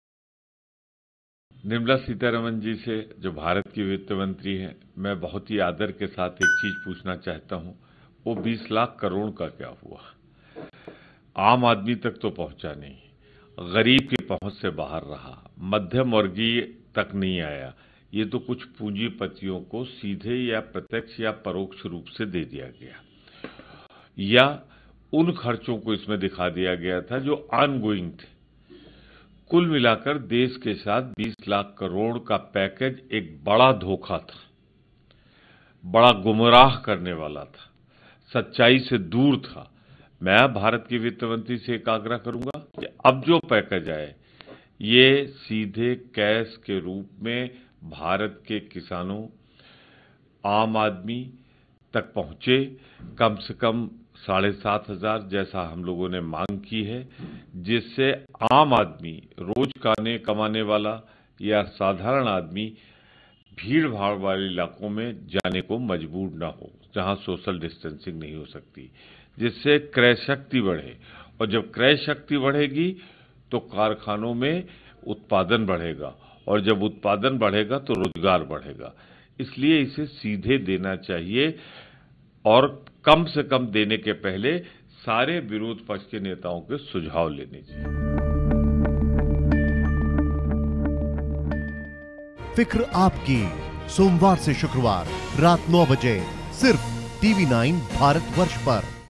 '20 लाख करोड़ के पैकेज को लेकर देश के साथ किया गया धोखा': Pramod Tiwari
कोविड- 19 महामारी (COVID-19 Pandemic) के बीच अर्थव्यवस्था को बूस्ट देने के लिए सरकार एक और राहत पैकेज की घोषणा जल्द कर सकती है. वित्त मंत्री निर्मला सीतारमण (Finance Minister Nirmala Sitharaman) ने कहा कि कोरोना संकट से पैदा हुए हालात से निपटने के लिए सरकार ने चालू वित्त वर्ष में तीसरे प्रोत्साहन पैकेज (Stimulus Package) के विकल्प को बंद नहीं किया है. अब कांग्रेस नेता प्रमोद तिवारी ने सरकार पर निशाना साधा है.